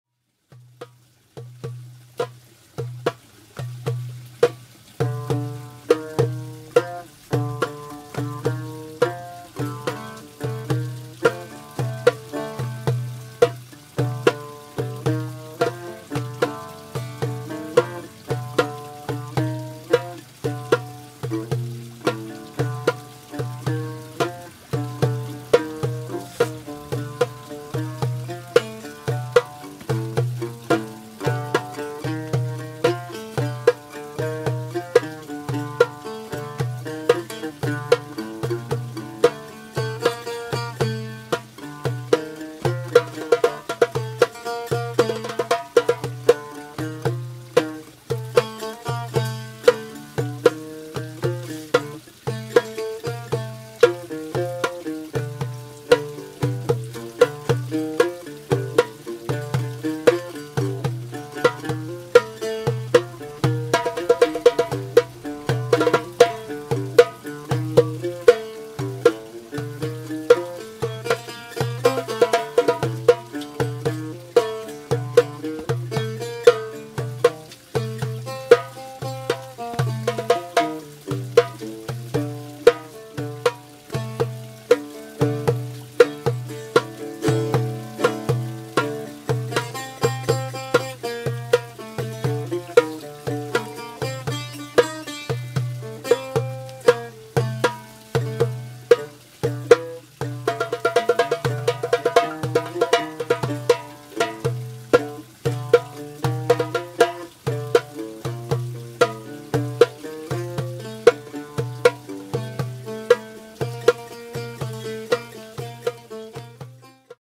Labanese Circle Dance " Dabke" by my fish pond
Rehearsal  with Taissia and Jack (Hagop) drumming Kenny on bouzouki
a Labanese dance style called "Dabke"  where everyone holds  hands or  arms on  shoulders to  dance in   circle  and  match the moves